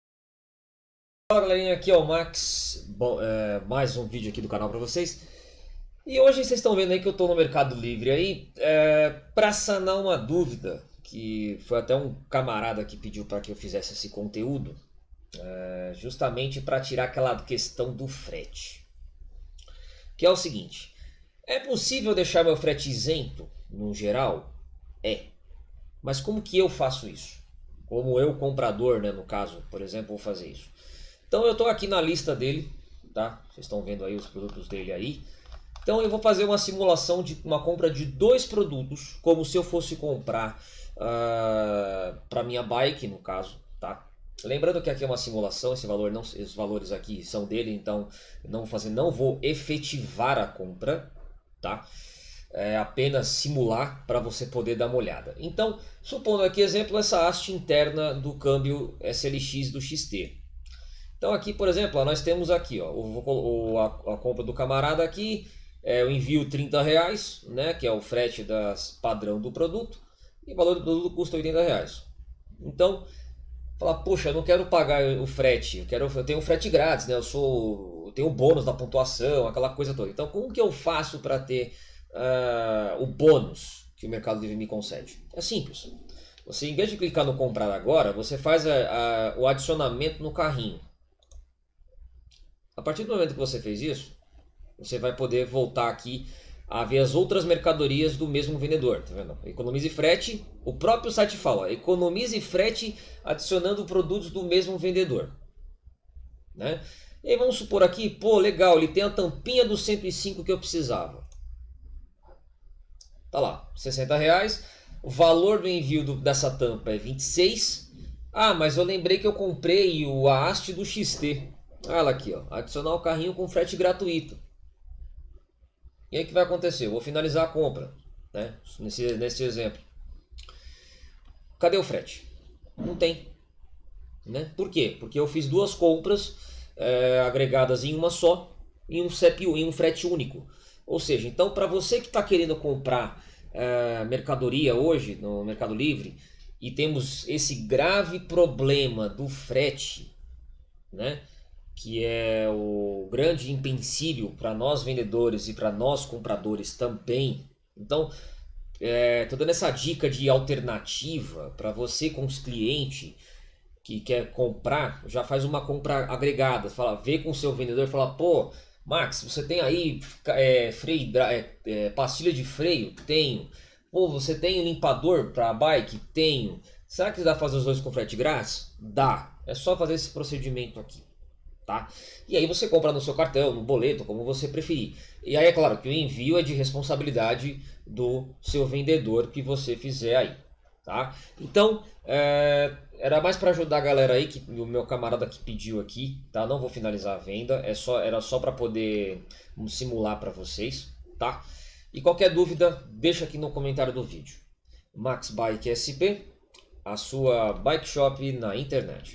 Ganhe Frete grátis em qualquer compra no mercado livre!!
Apoie o Canal para ele manter-se funcionando

No vídeo de hoje é apenas uma explanação sobre a compra de produtos pequenos para obtenção de frete grátis para sua cidade.